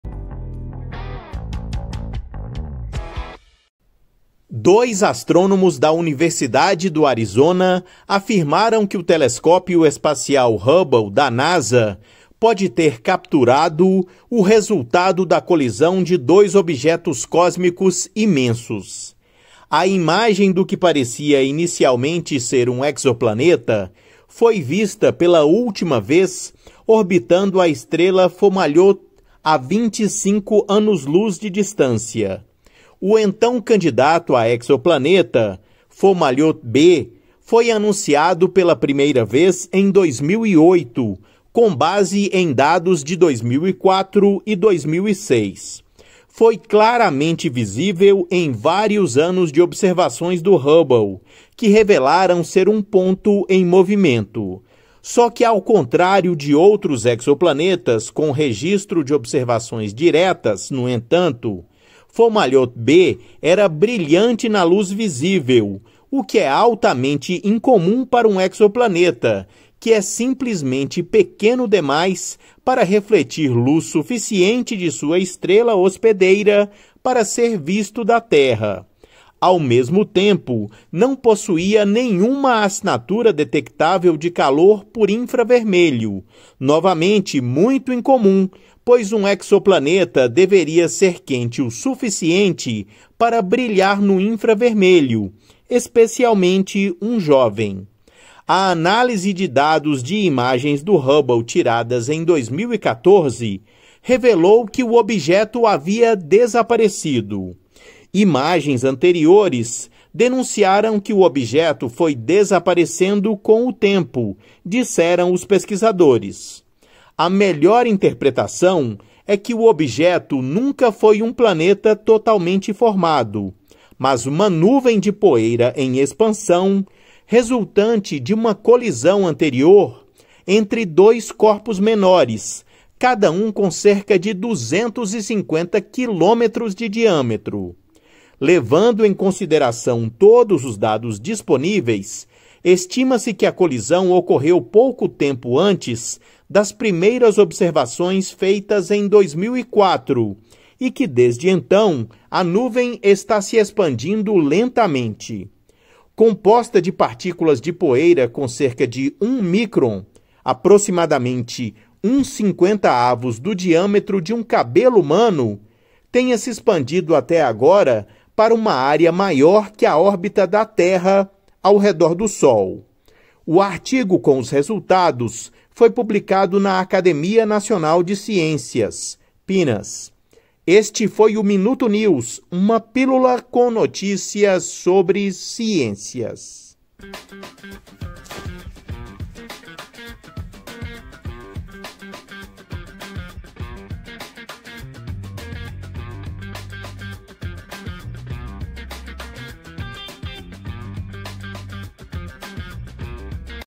HUBBLE REGISTRA RESULTADO DE COLISÃO GIGANTE !!!
Dois astrônomos da Universidade do Arizona afirmam que o Telescópio Espacial Hubble da NASA pode ter capturado o resultado da colisão de dois objetos cósmicos imensos.

A imagem do que parecia inicialmente ser um exoplaneta foi vista pela última vez orbitando a estrela Fomalhaut, a 25 anos-luz de distância.

O então candidato a exoplaneta Fomalhaut b, foi anunciado pela primeira vez em 2008, com base em dados de 2004 e 2006.

Foi claramente visível em vários anos de observações do Hubble, que revelaram ser um ponto em movimento.

Só que ao contrário de outros exoplanetas com registro de observações diretas, no entanto,  Fomalhaut b era brilhante na luz visível - o que altamente incomum para um exoplaneta, que é simplesmente pequeno demais para refletir luz suficiente de sua estrela hospedeira para ser visto da Terra.

Ao mesmo tempo, não possuía nenhuma assinatura detectável de calor por infravermelho - novamente, muito incomum, pois um planeta deveria ser quente o suficiente para brilhar no infravermelho, especialmente um jovem .

A análise de dados de imagens do Hubble tiradas em 2014 revelou que o objeto havia desaparecido.

Imagens anteriores revelaram que o objeto foi desaparecendo com o tempo, disseram os pesquisadores

 A melhor interpretação é que o objeto nunca foi um planeta totalmente formado, mas uma nuvem  de poeira em expansão resultante de uma colisão anterior entre dois corpos menores, cada um com cerca de 250 quilômetros de diâmetro.

Levando em consideração todos os dados disponíveis, estima-se que colisão ocorreu pouco tempo antes das primeiras observações feitas em 2004 e que desde então a nuvem está se expandindo lentamente.

composta de partículas de poeira com cerca de 1 mícron  cerca de 1/50 do diâmetro de um cabelo humano - tenha se expandido até agora para uma área maior que a órbita da Terra ao redor do sol.

O artigo com os resultados foi publicado na Academia Nacional de Ciências (PNAS).

Credito imagens:

Canal HubbleESA
NASA